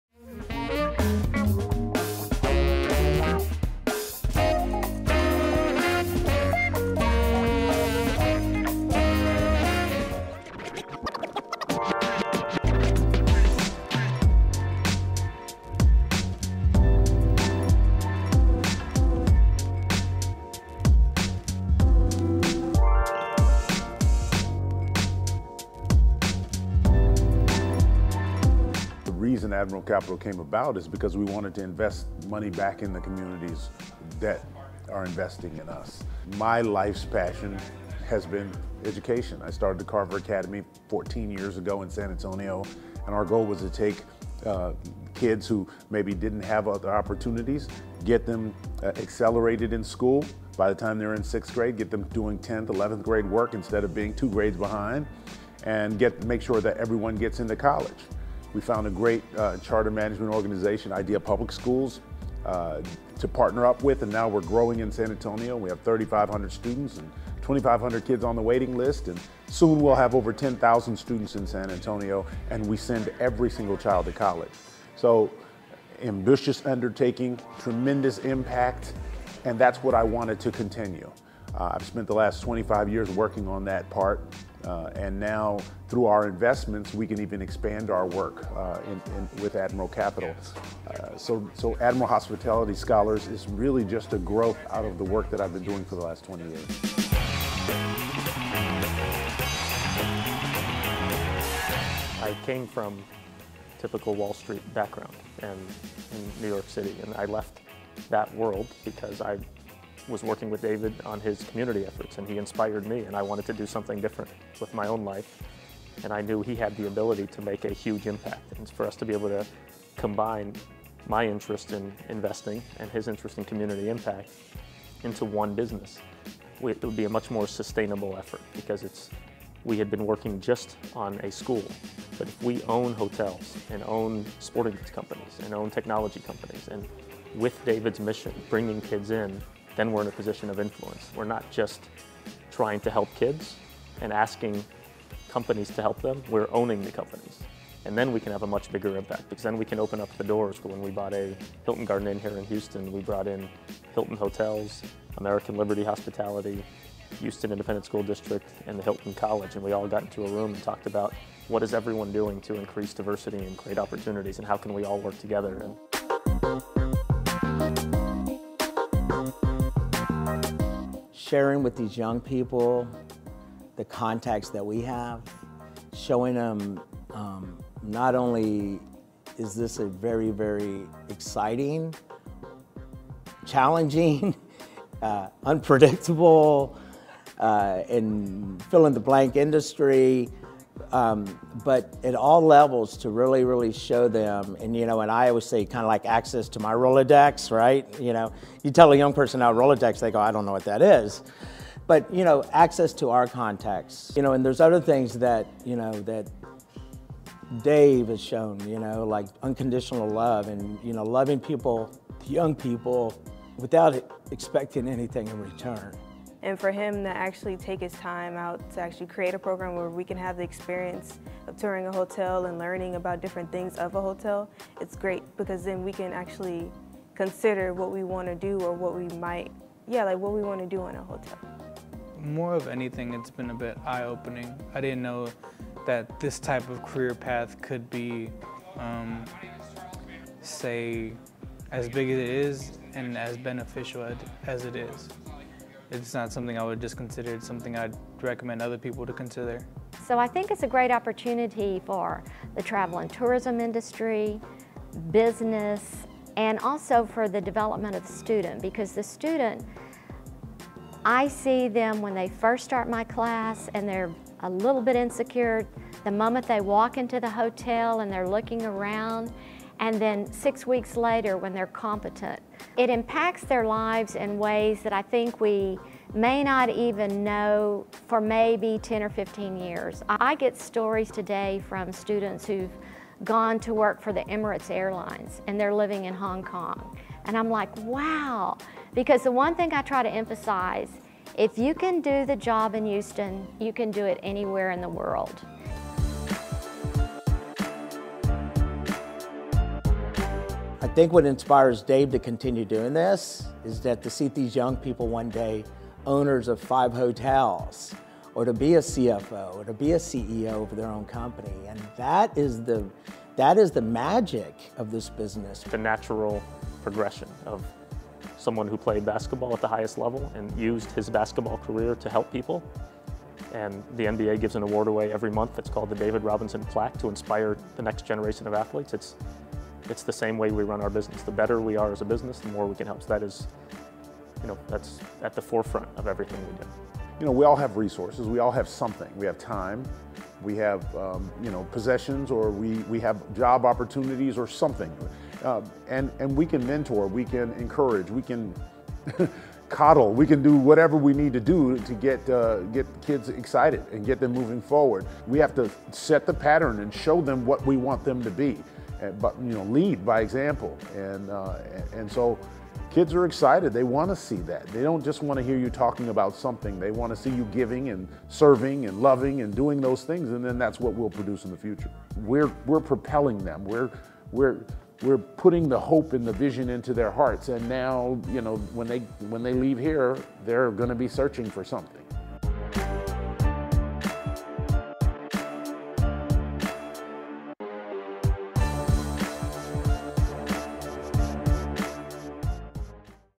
Admiral Investments
David Robinson explains his investment program that nurtures, and develops, young people for careers in the hotel industry